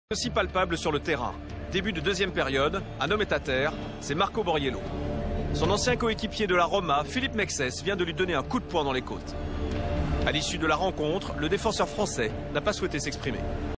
Mexes Give A Punch To Boriello
Beginning of the second half-time, a man is down, it was Marco Boriello. His former teammate of Roma Philippe Mexes just give him a punch in the ribs. At the end of the match the French defender didn't want to comment.